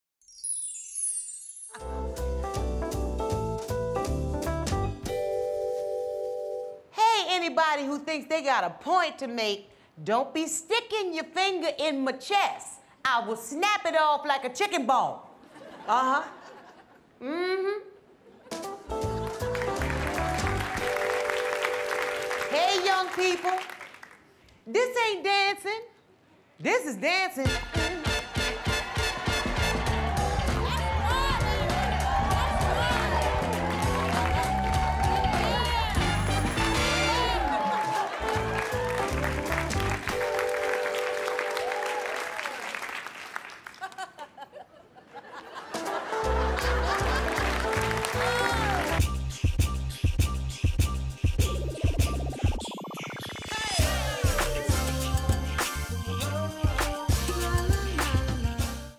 FYI with Esther Jean Johnson: Breaking Fingers, Dance Moves and Hand Jives
Your favorite Aunt, Esther Jean Johnson weighs her opinion on various topics.